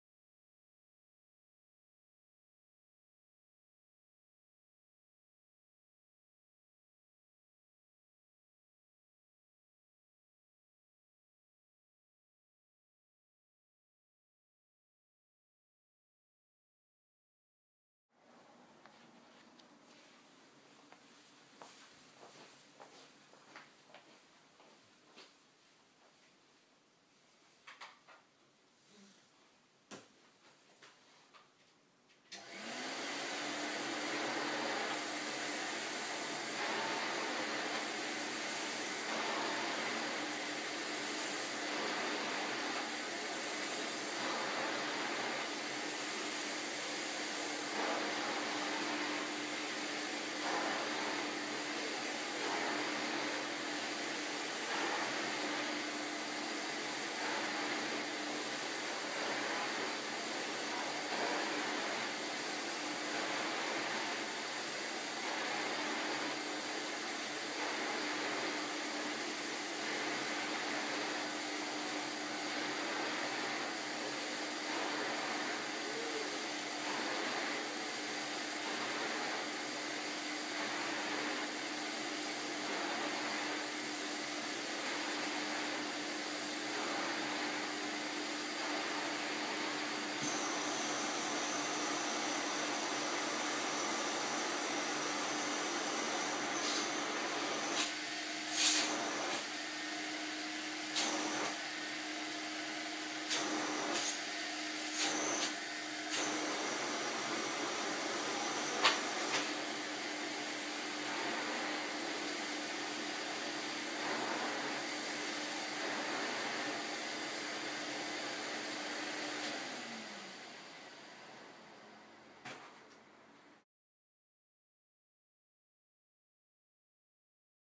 1985 Kerstar KV15/1 commercial vacuum
Kerstar is a very well established maker of Commercial and industrial vacuums. They are based in Northampton, England and the quality and reliability of their cleaners is legendary. They've been around for 50 odd and are still going strong.

This is my KV15/1 from 1985. I rescued this one from a vacuum shop, it had been sitting in there for years. Prior to that it was owned by a convent apparently. With a bit of tlc i've got it working beautifully again. This was one of the larger models in the commercial range, there was a smaller version that is still made to this day virtually unchanged. It's a simple but rugged machine and believe it or not, has a hepa filter under the chrome exhaust outlet on top. It takes standard Numatic Henry bags. A great commercial vacuum.